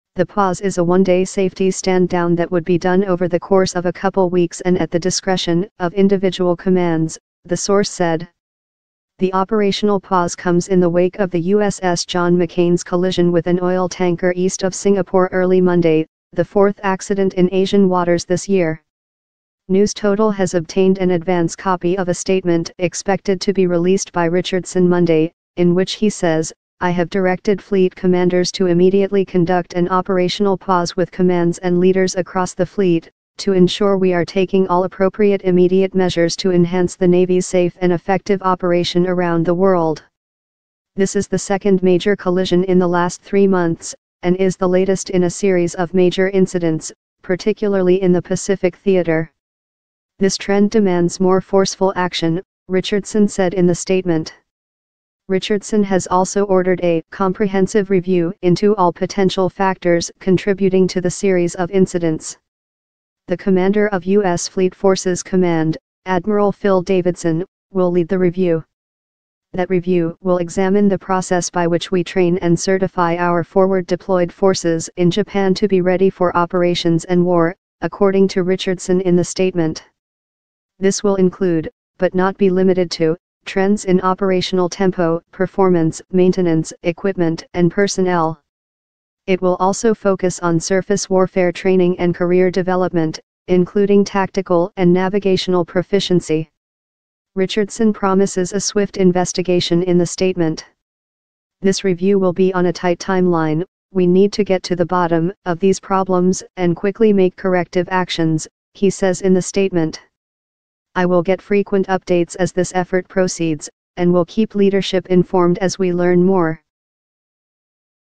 US Navy Plans 1day Operational Pause Following Warship Collisions Politics
The pause is a one-day safety stand-down that would be done over the course of a couple weeks and at the discretion of individual commands, the source said.